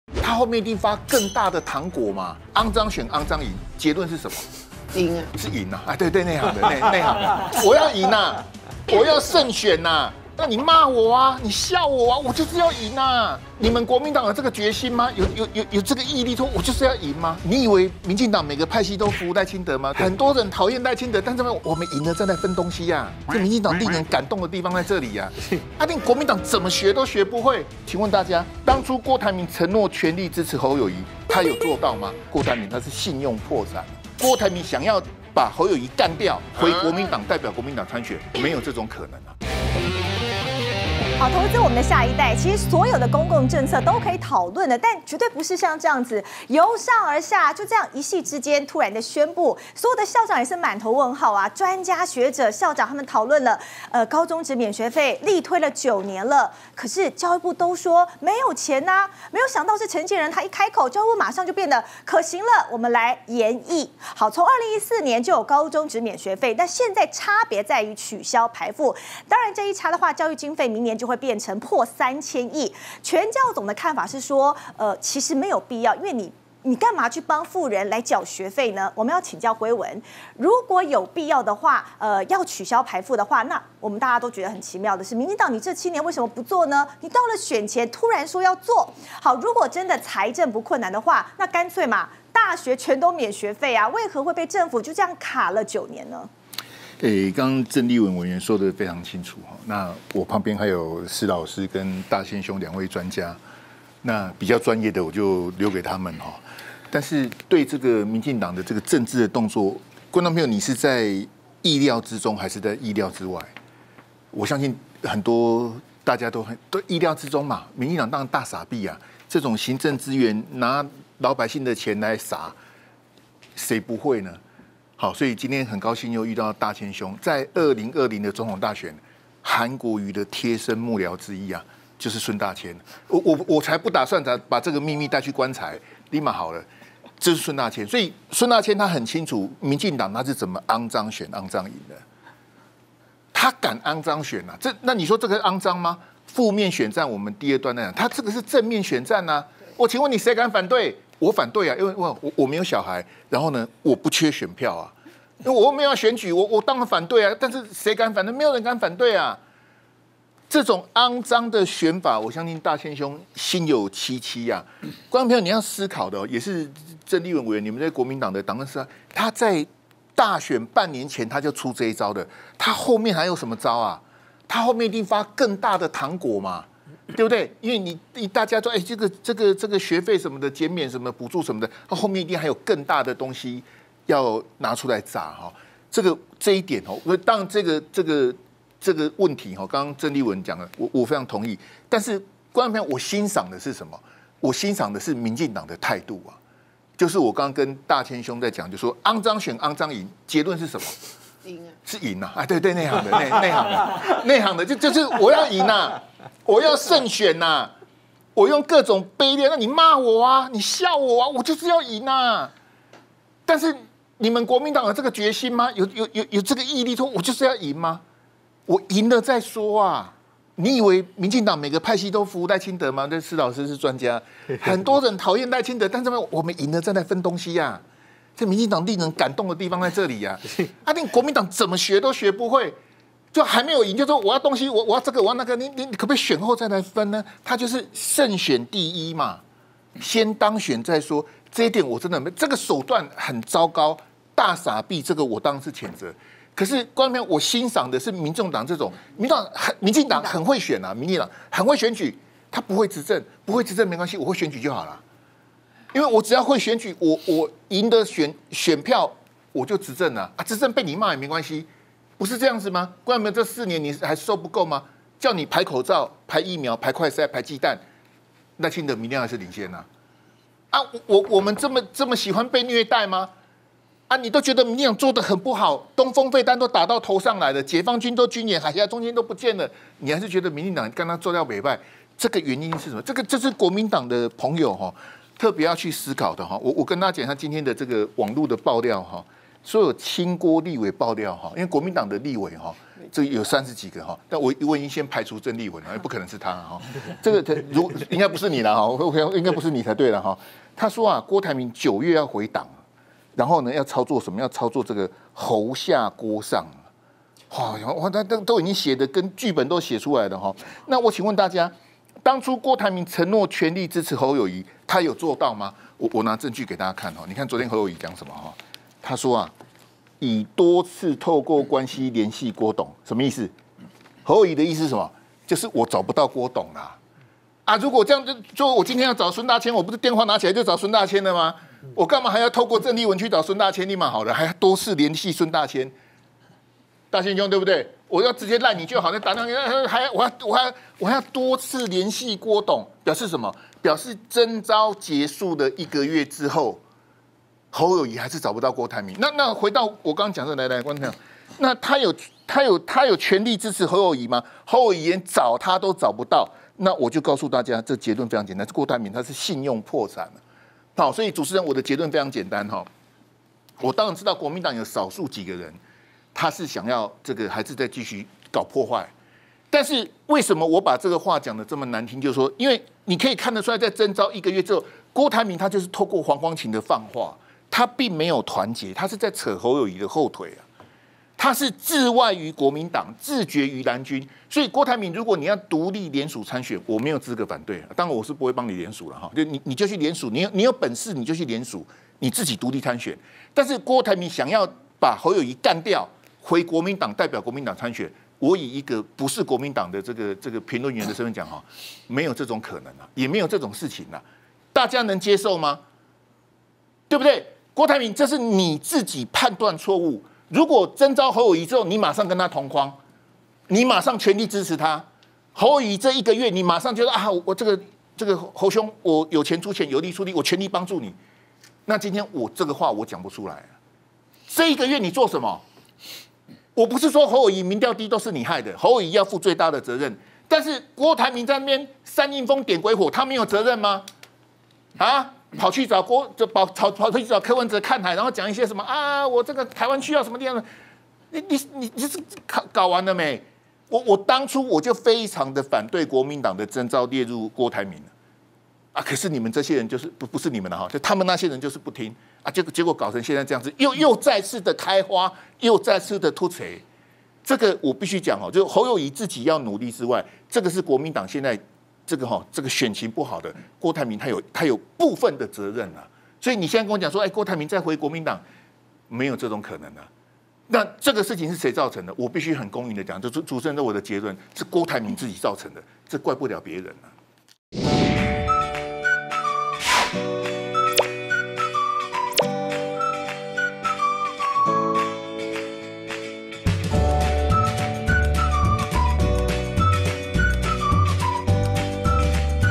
批綠"骯髒選骯髒贏"!私校補助遭轟"大撒幣"..陳揮文嘆:為何藍營總學不會?
責任小編:懸編/肉肉編